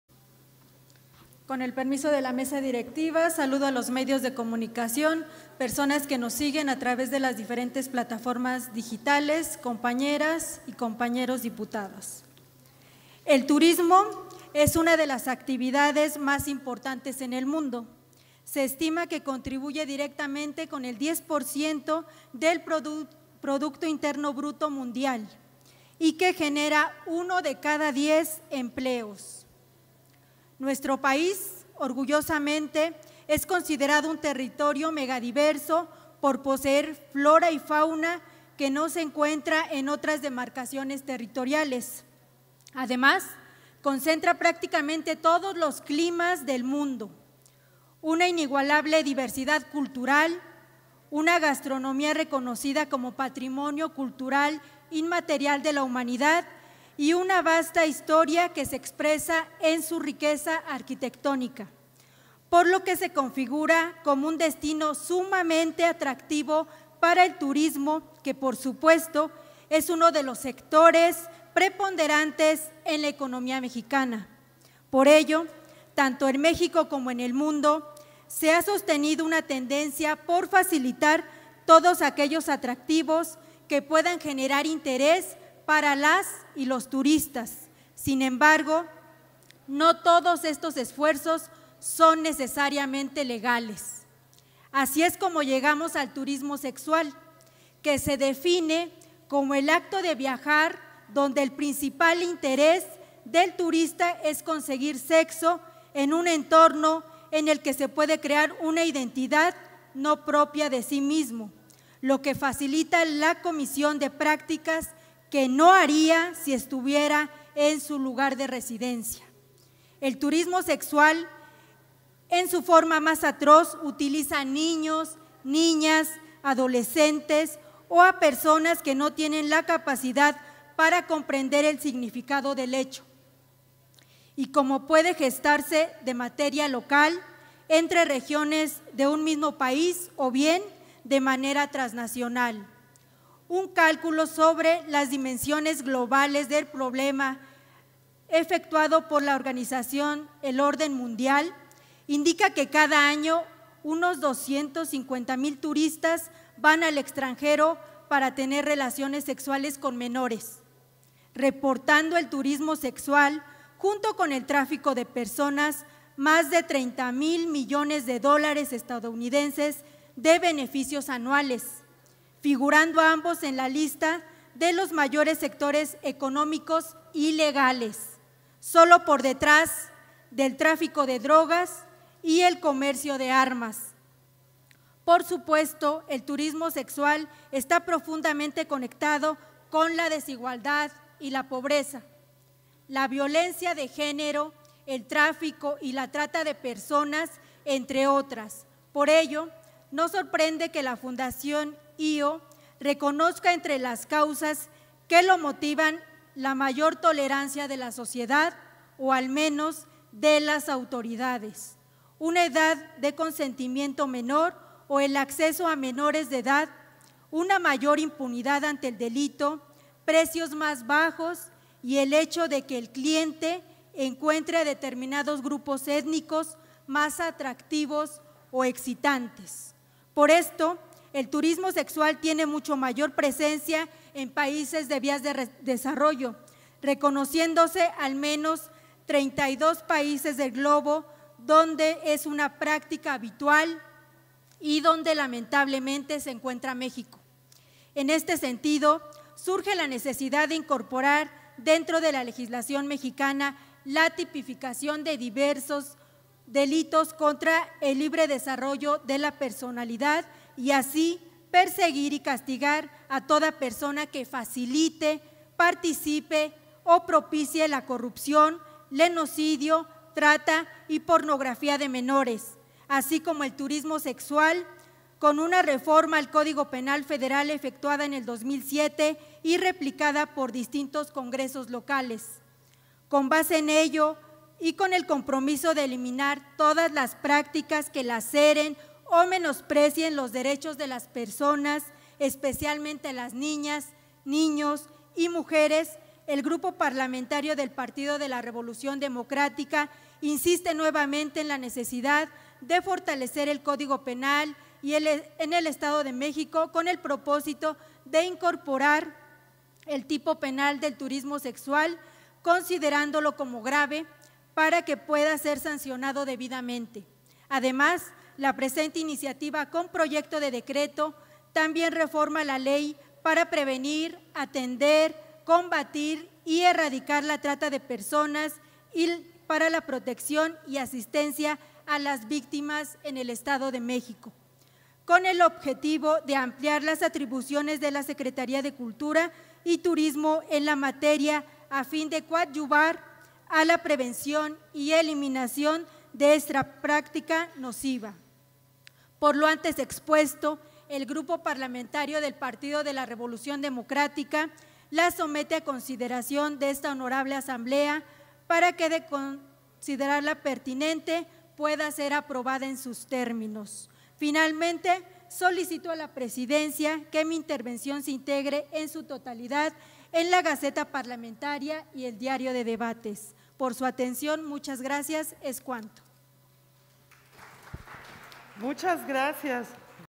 9/12/2021: Intervención Dip. Viridiana Fuentes
Sesión deliberante, Primer Periodo Ordinario, 1er. Año, LXI Legislatura Mexiquense